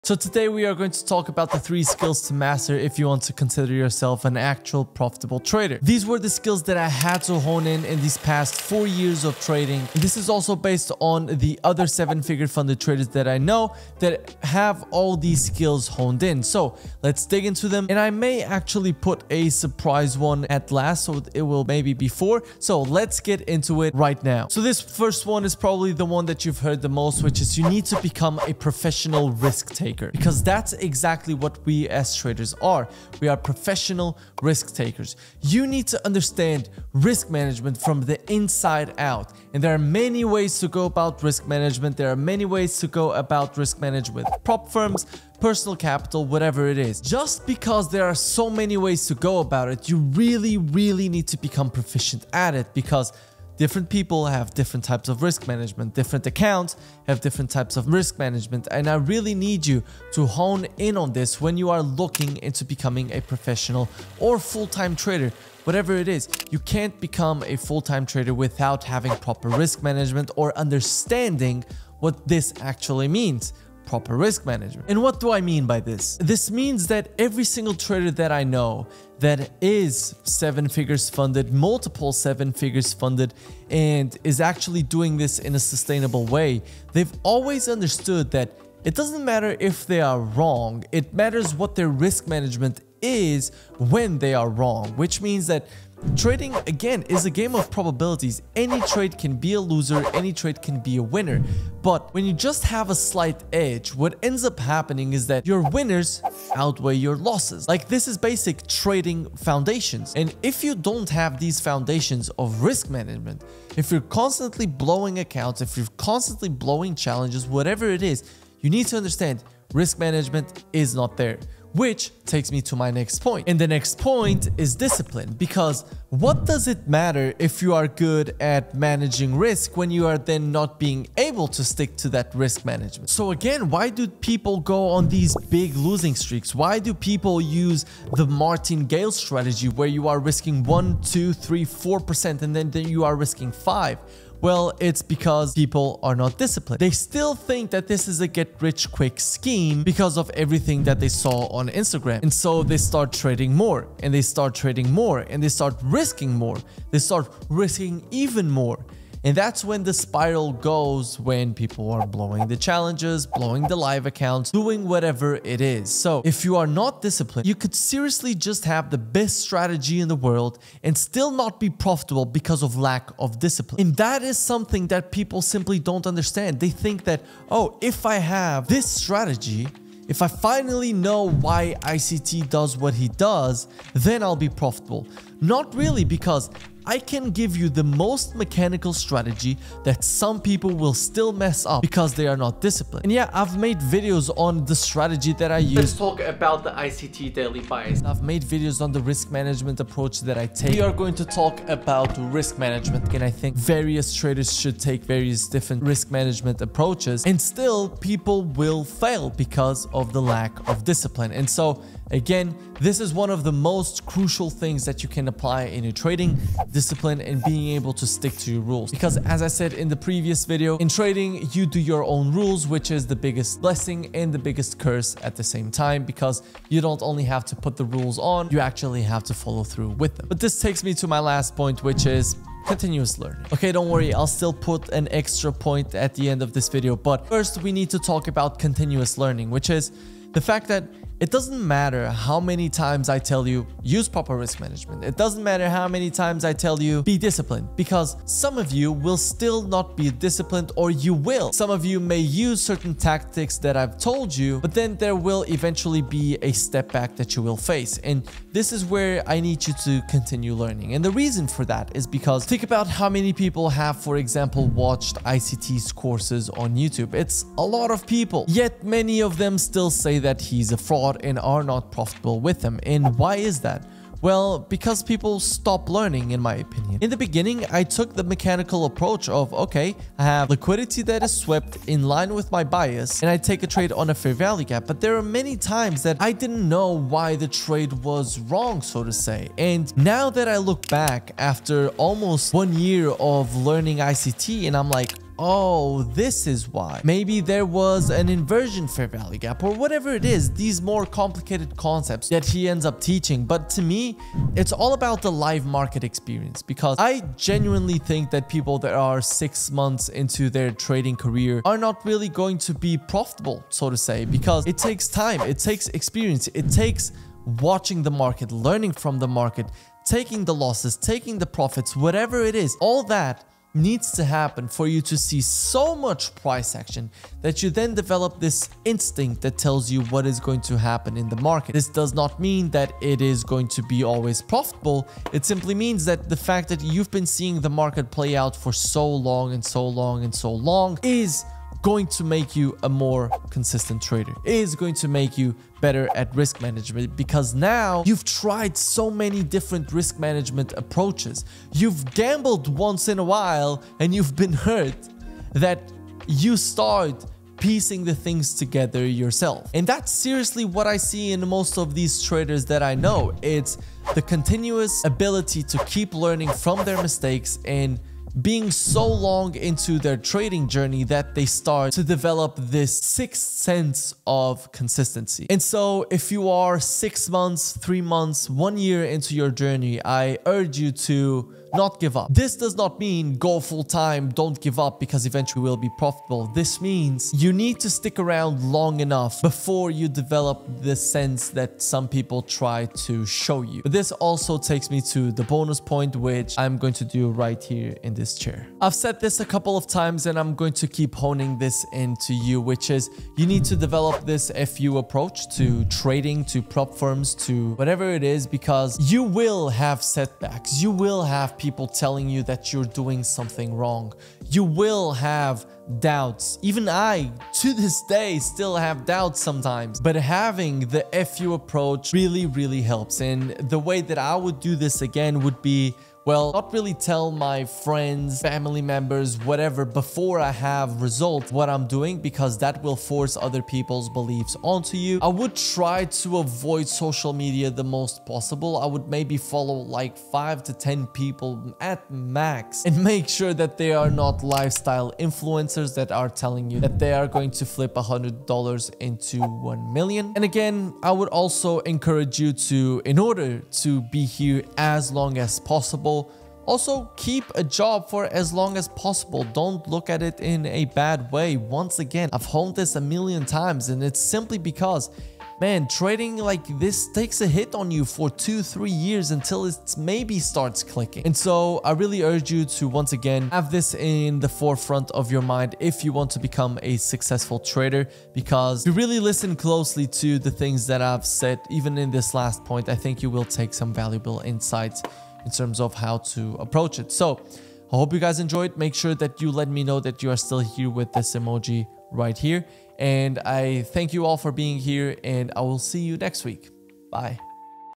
3 SKILLS to become a SUCCESSFUL TRADER
Let's talk about what you need to become a successful funded trader.

📈 Trading Platform (30$ off with this link)